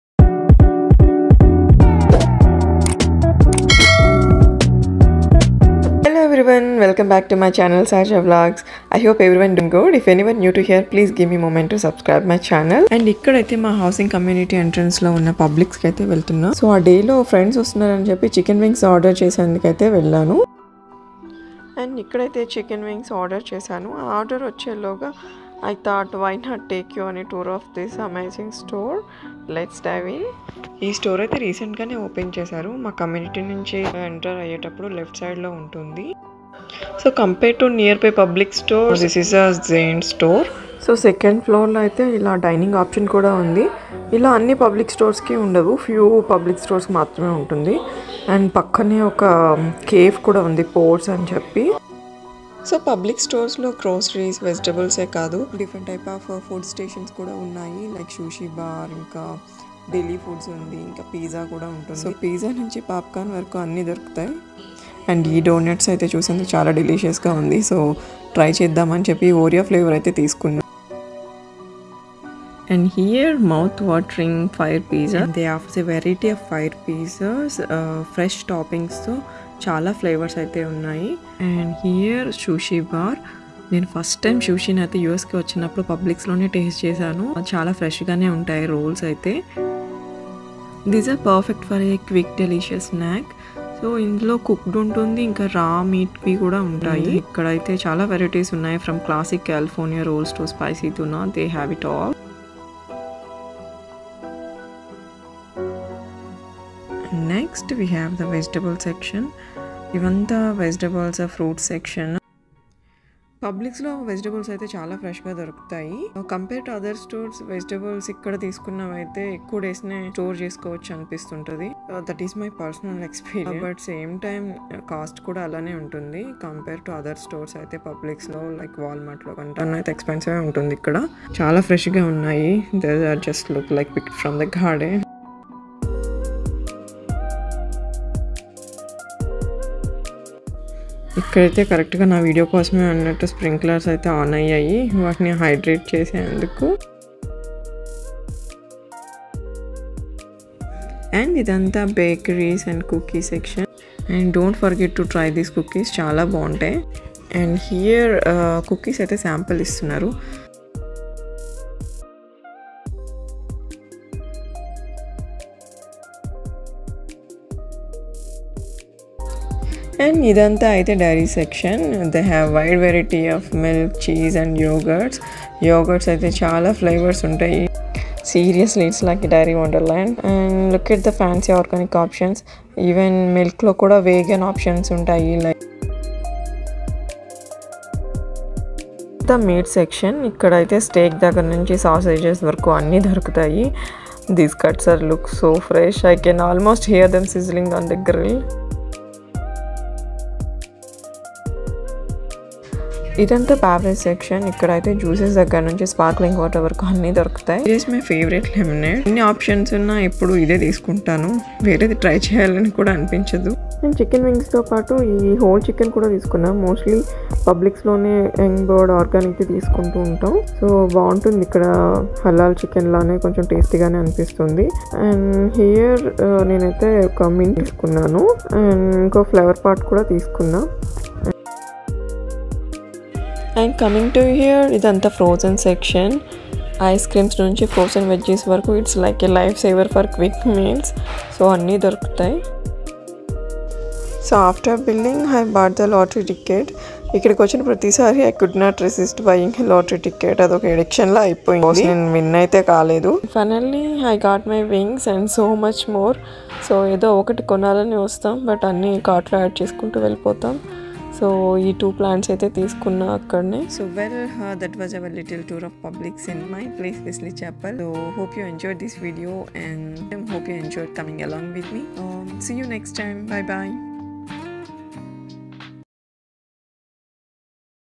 Gaint publix in tampa Florida | Sahaja Vlogs | telugu
Gaint Publix Store Adventure: Spicy Chicken Wings, Lottery Tickets & More!
Exploring TOP Giant Publix: Spicy Wings & Lottery Tickets!

Publix Store tour | Sahaja Vlogs | telugu

Hey everyone! Join me on today’s daily vlog as I take you on a fantastic tour of a Giant Publix store! I went in for some spicy chicken wings and a lottery ticket, but I ended up capturing so much more. From fresh groceries and colorful vegetables to delicious donuts, sushi, and fire pizza - I’ve got it all covered! 🚶‍♂️

This video will give yoismartshanku a sneak peek into the diverse food stalls and  items available in Publix, letting our international friends see what stores in the USA are like.